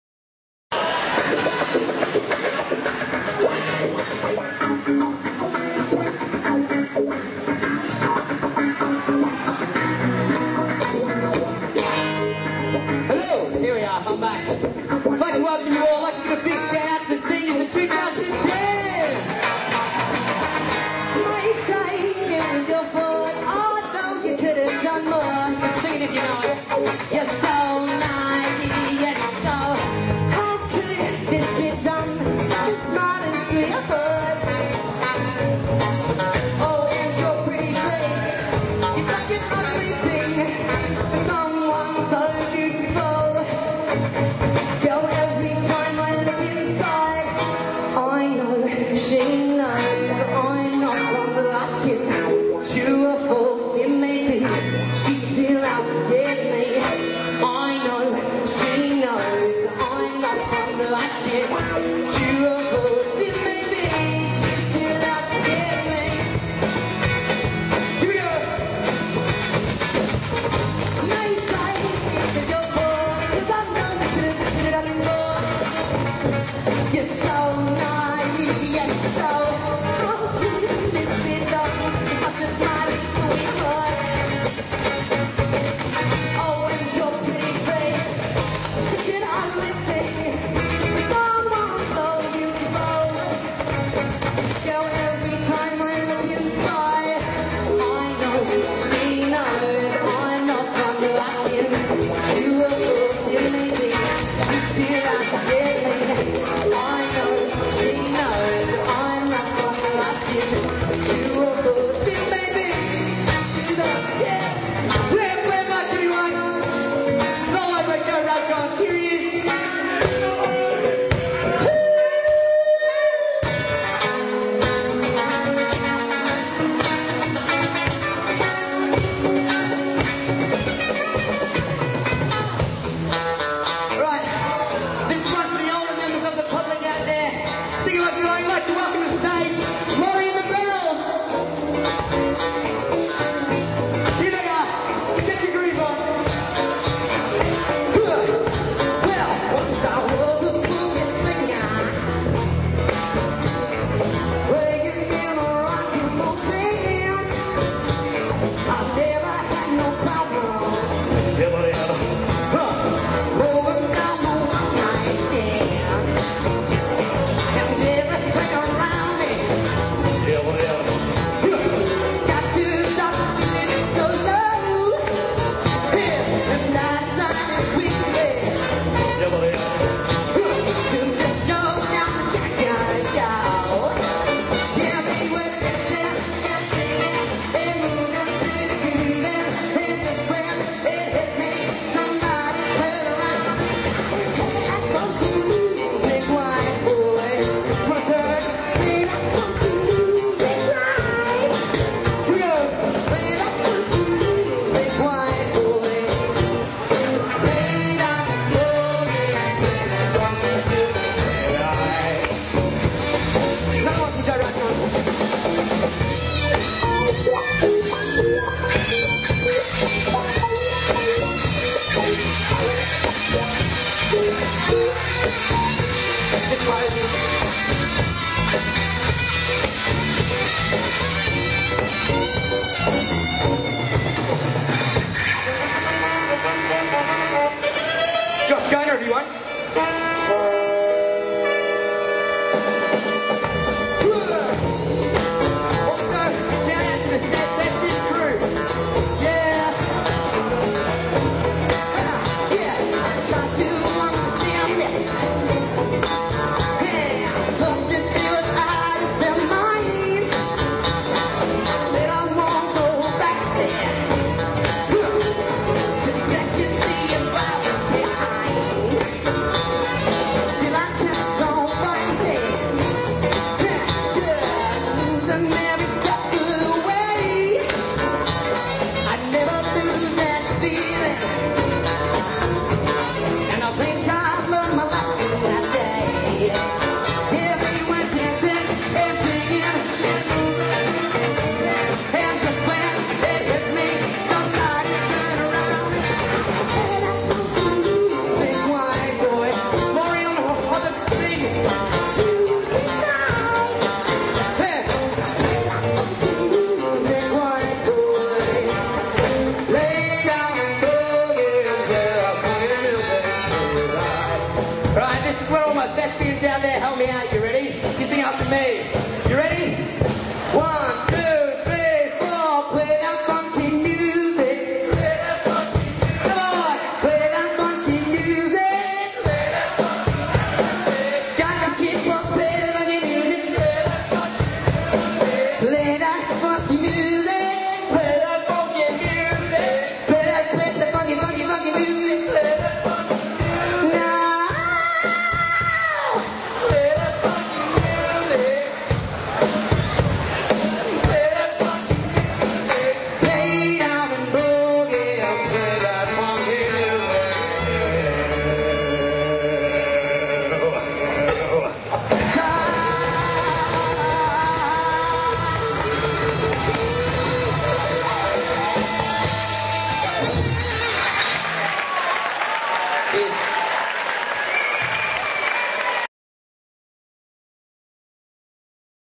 Heaven On a Stick - Battle of the Bands 2010
A glorious day of music where Heaven On a Stick featured in the 2010 Battle of the Bands with the likes of Plagued Youth and Gravity Scam.